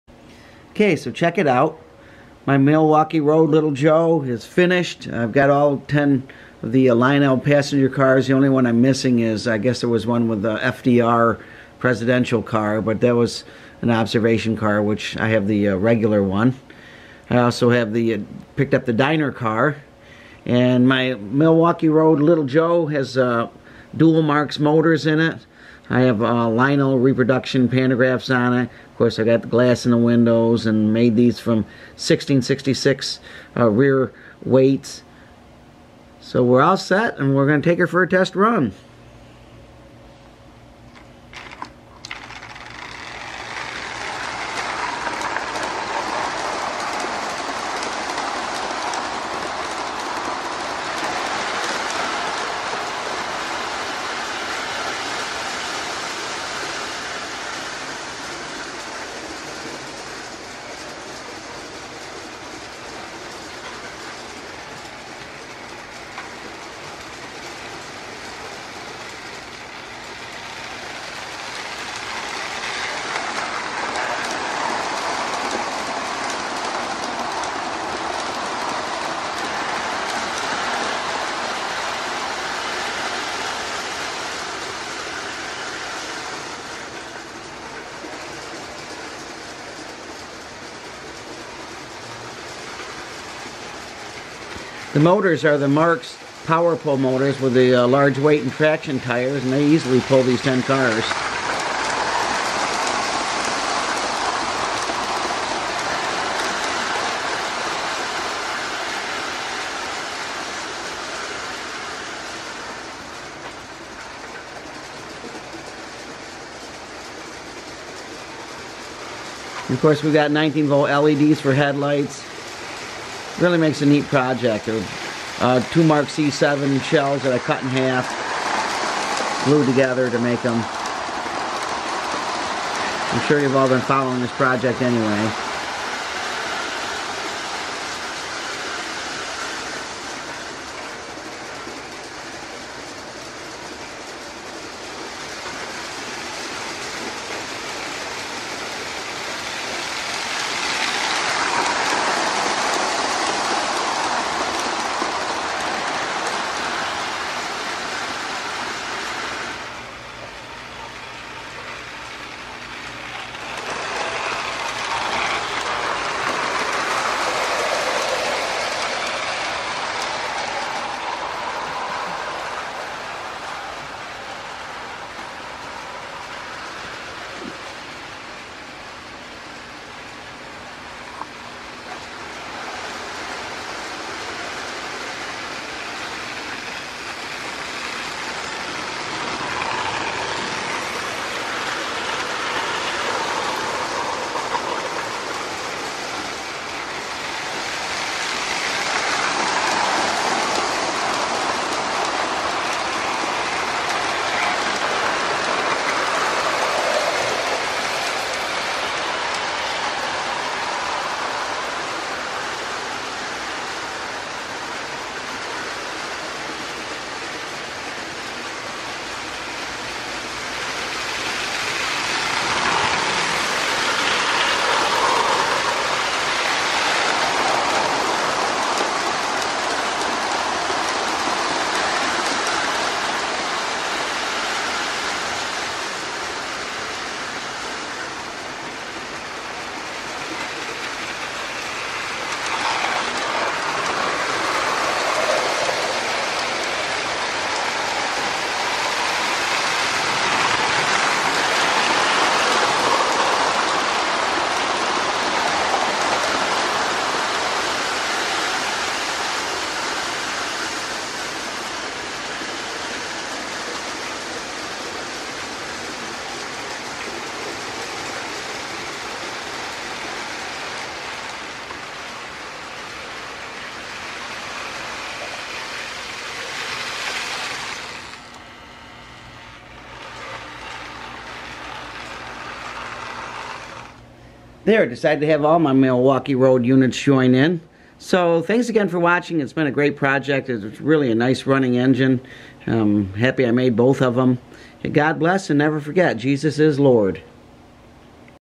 Marx Milwaukee Mayhem! Little Joe passenger & freight
Finally completed my collection of Lionel baby madison cars with the purchase of the City of Milwaukee & Diner cars. My dual powered Marx little joe easily handles them and a pair of custom E7s and the freight little joe join in! Dual powered unit has two E7 power pull motors and runs smooth! Both are wired to one E unit.